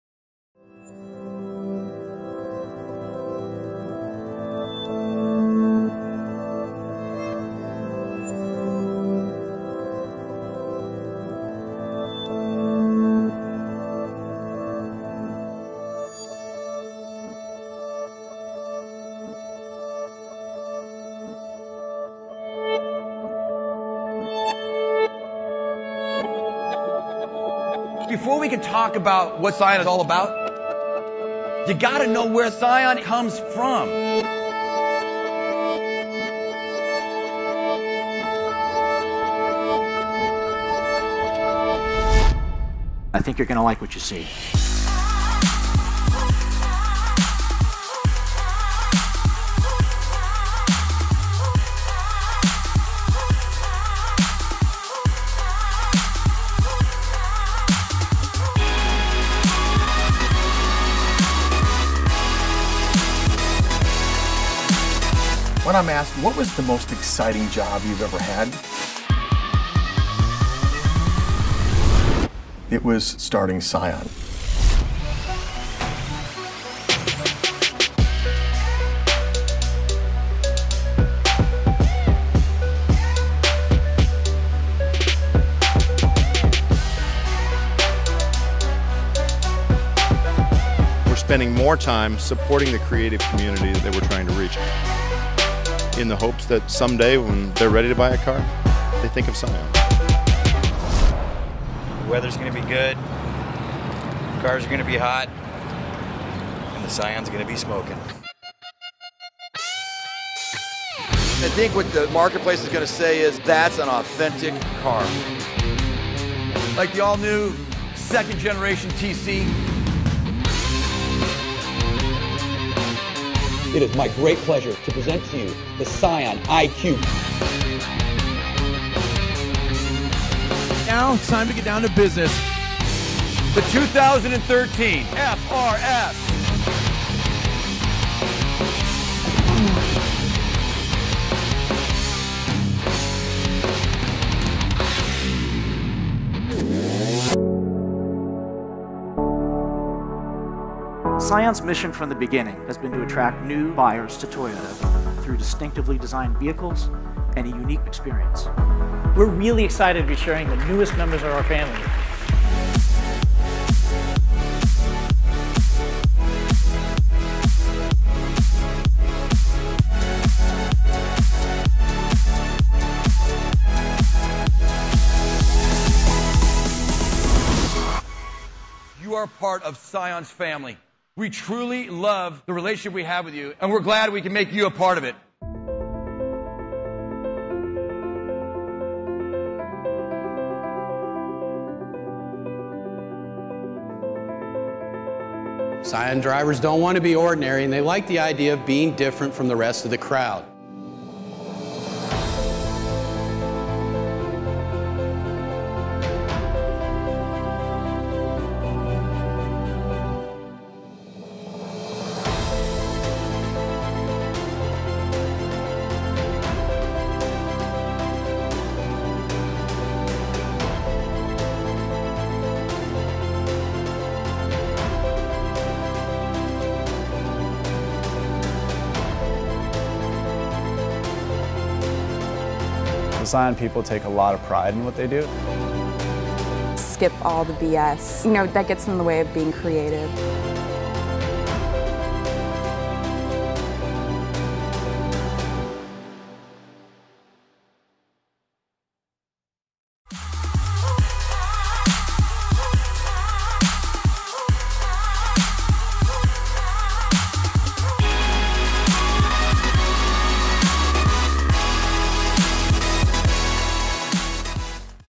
Scion Lives On
Let’s celebrate the good times with a look back on 13 groundbreaking years, starting with the one that started it all – the 2004 Scion xB.

Scion was established in 2003 as a test laboratory division for Toyota Motor Sales. It was designed to offer products and processes that stood apart from the crowd. Scion offered distinctive vehicles that reflected owners’ passions. Scion also appealed to a younger audience by offering a simple, no-haggle buying experience and by engaging with potential customers in meaningful and creative ways. to learn more.